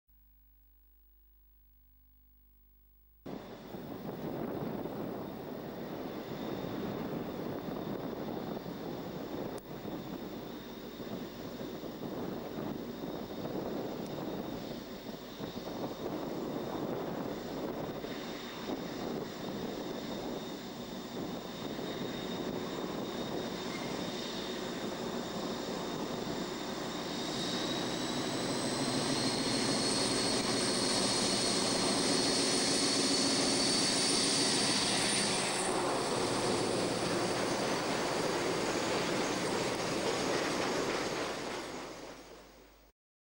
Class 56 No.56060 'The Cardiff Rod Mill' hauls a coal train up Stormy down Bank.
Class 56 No. 56060 named 'The Cardiff Rod Mill' hauls a coal train up Stormy down Bank in Mid 1990's.